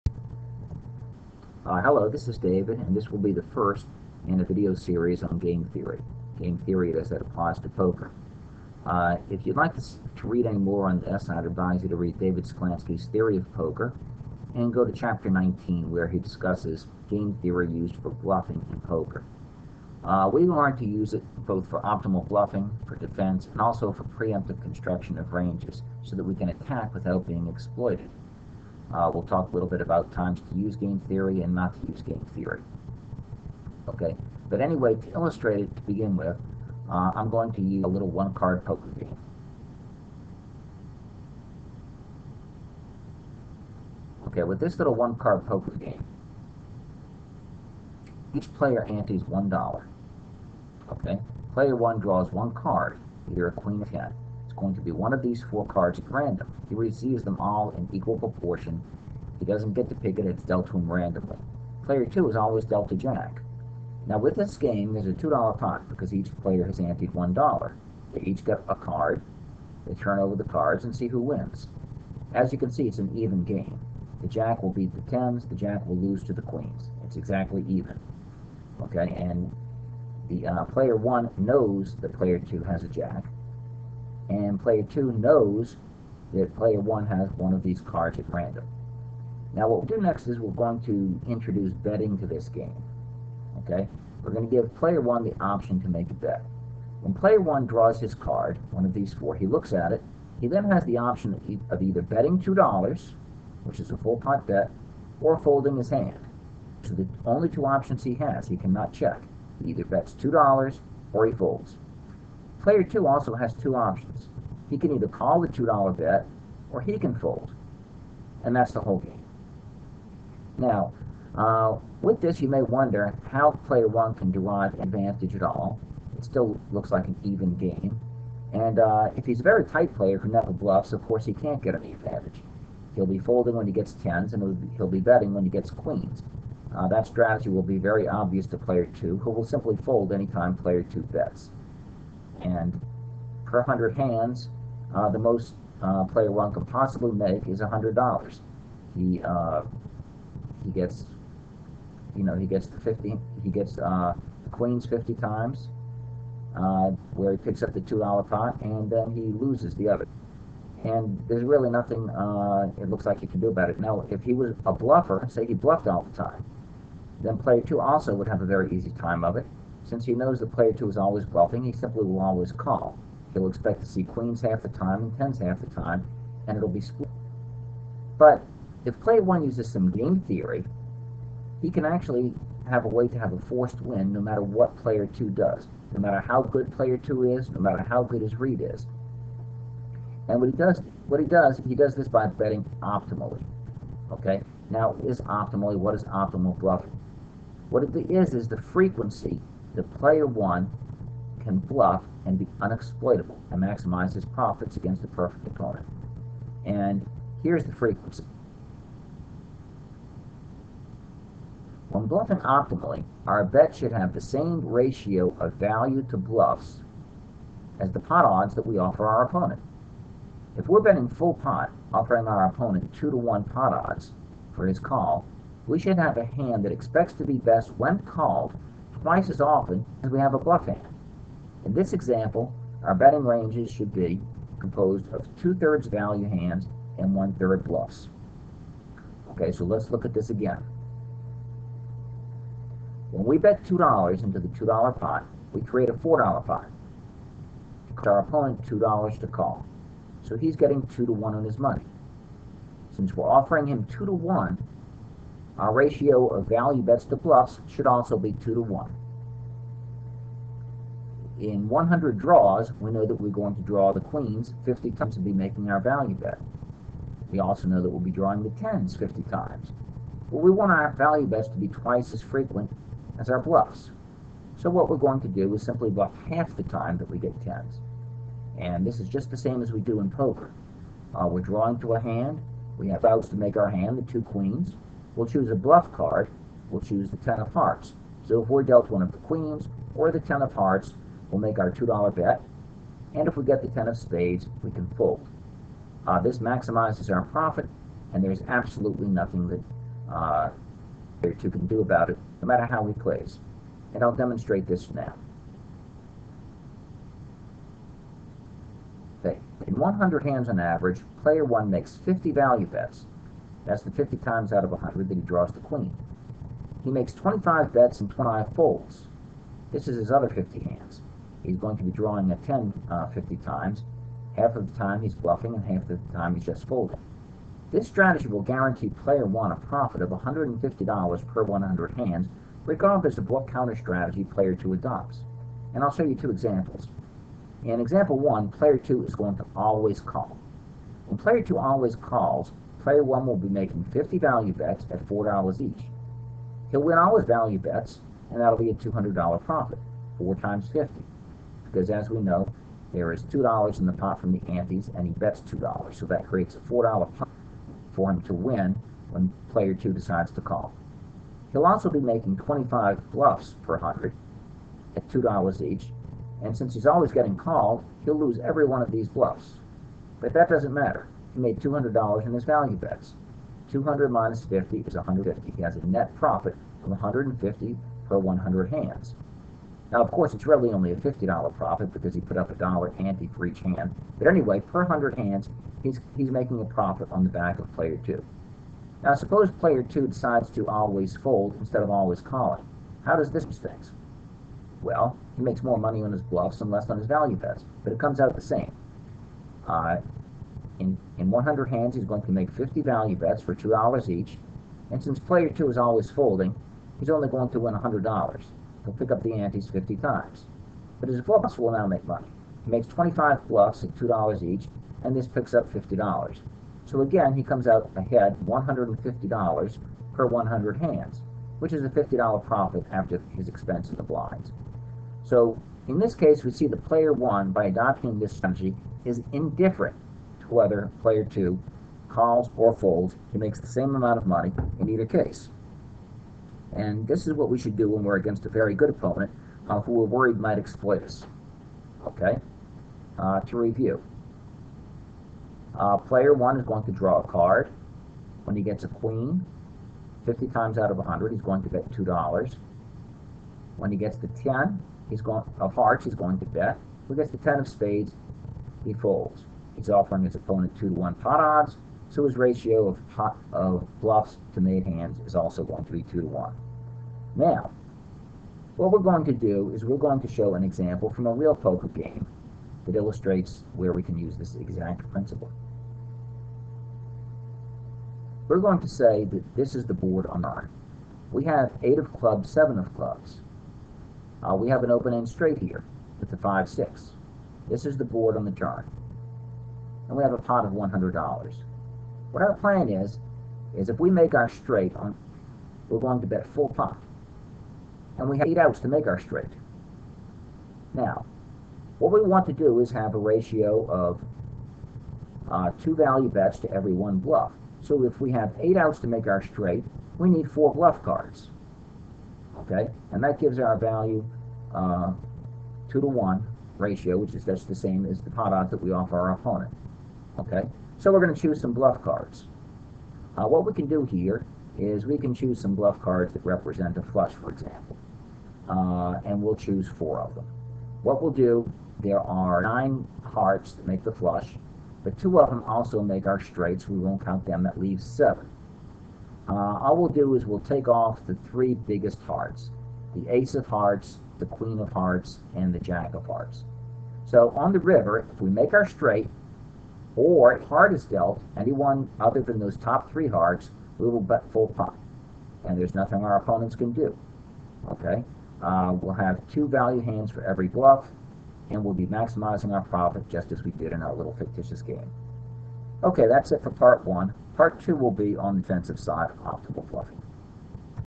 GameTheory Poker 1.wmv
Elementary Game Theory concepts discussed. Examples are shown using a one card poker game and then a last example that illustrates use while playing a straight draw in a holdem game